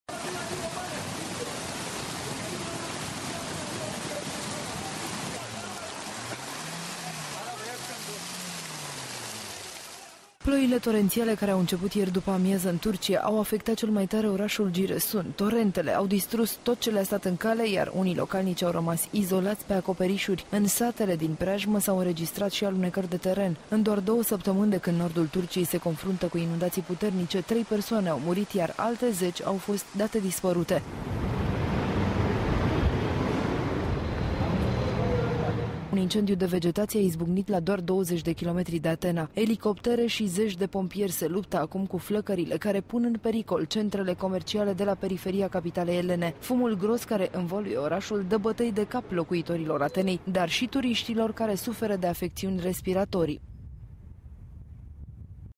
Inundaţiile şi incendiile au afectat mai multe localităţi
Inundaţiile şi incendiile au afectat mai multe localităţi din Turcia şi Grecia
Sute de locuinţe au fost inundate în nord-estul Turciei